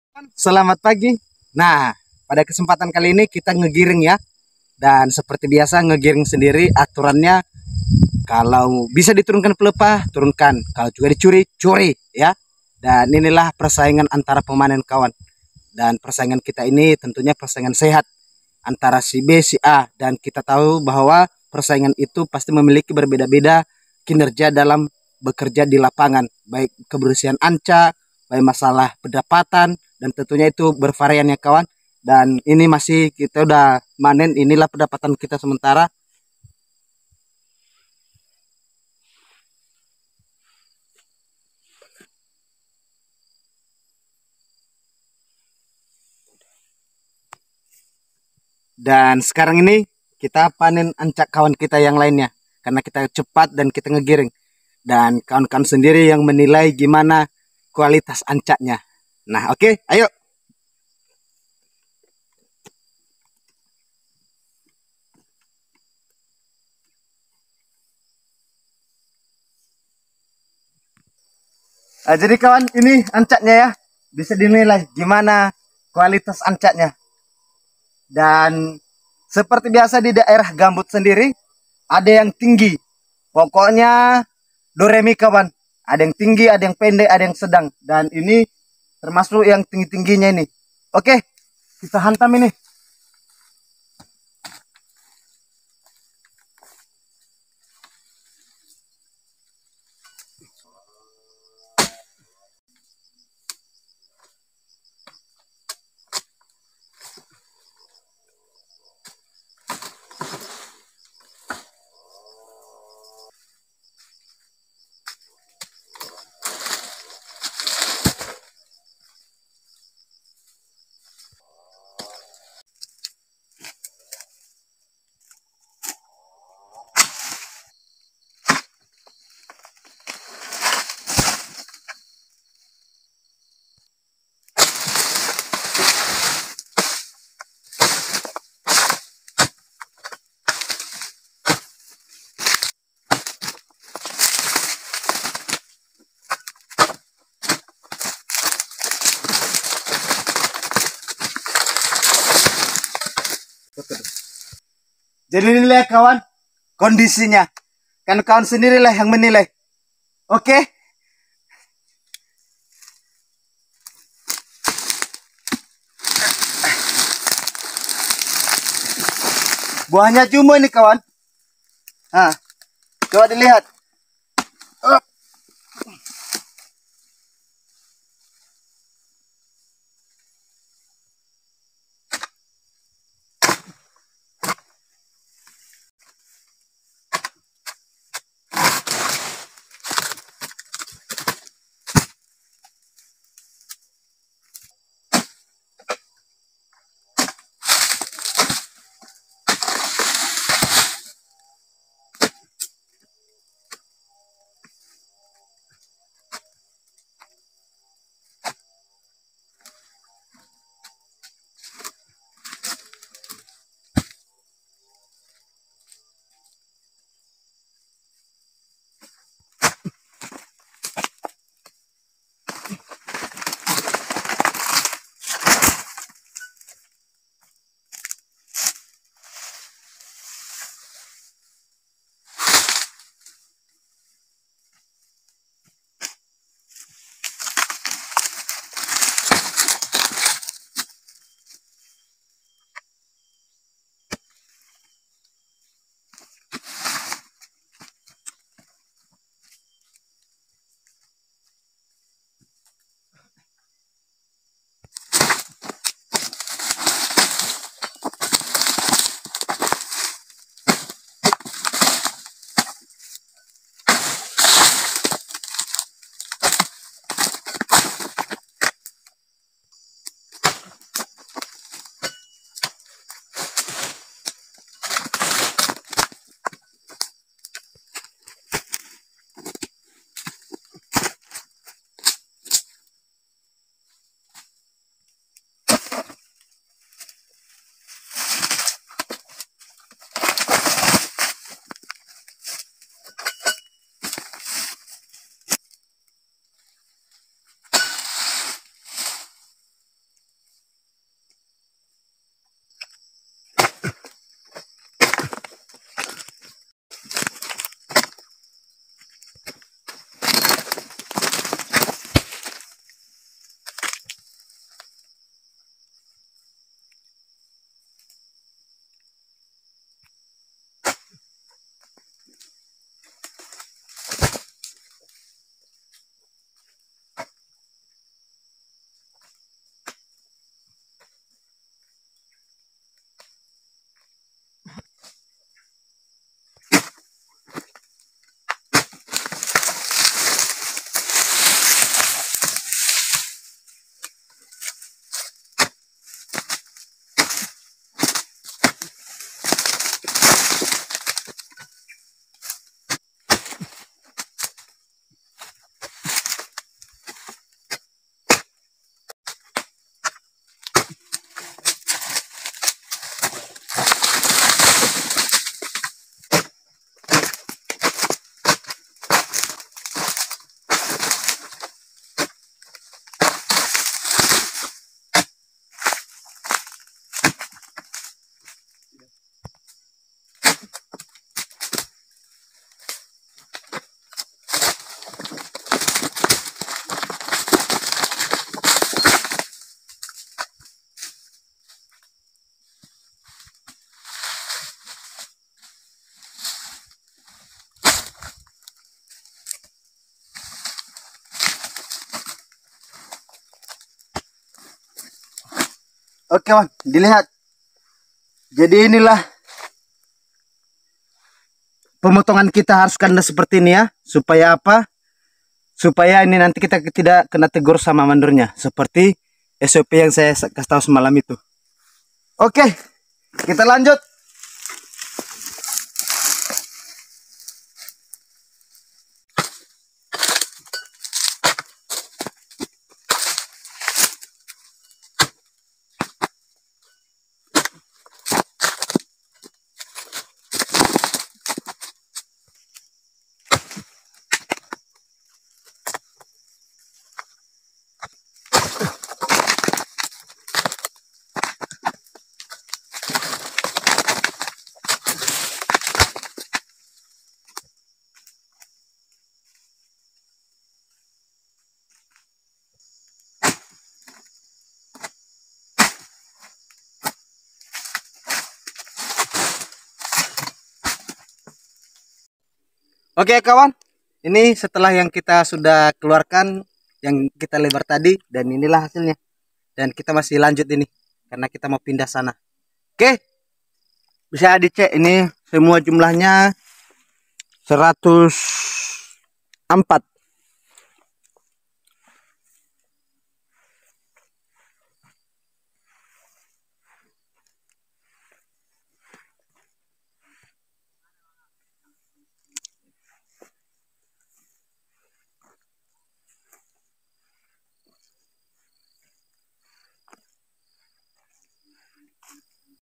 panen pokok tinggi hanya menggunakan#dodos super tajam
vidio panen sawit pokok tinggi cuman pakai dodos#dodos super tajam#panen sawit yangambi#cara menyusun buah di TPH#cara panen pokok tinggi